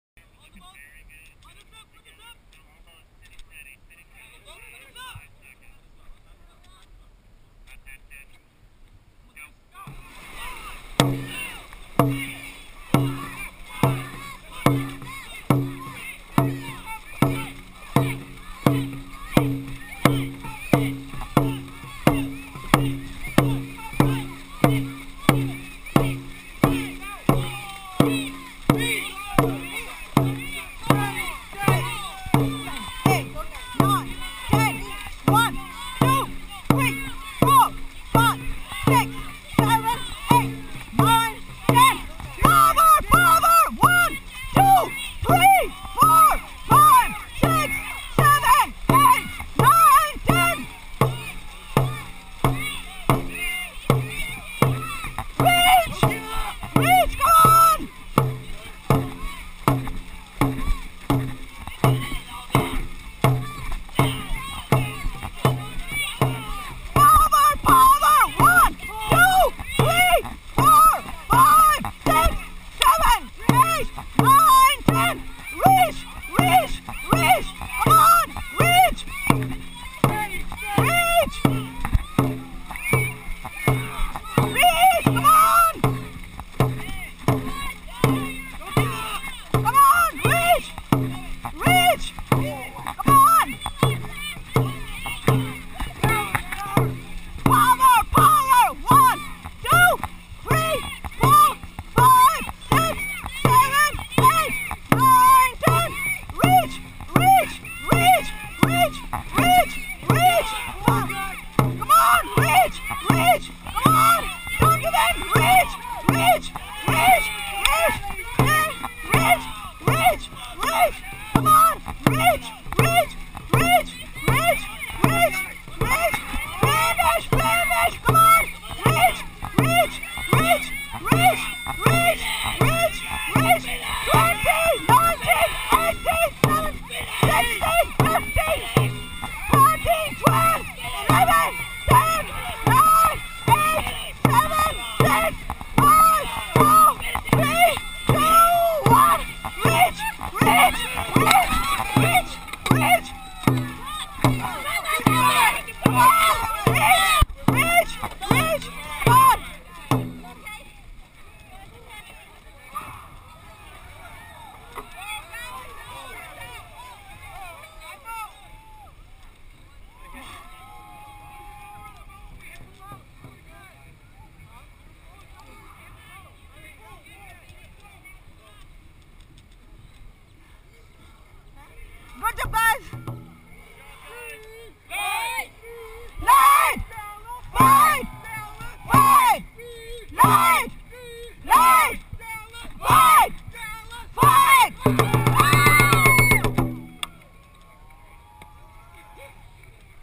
2013 Austin Race Final Heat
Our third and final heat at the 2013 Austin Dragon Boat Festival, racing for Championship.

This is DUC Delite's first time to be competing in the Competitive Category at the Austin race. We were racing against Houston Heat, Samsung Fire, Austin Coolers, and Team TORA. Competition was neck and neck, we were not able to tell how we placed until the awards ceremony.

The final outcome: DUC Delite snatched the overall Third Place, TORA won Second Place, and Houston Heat took home the Championship.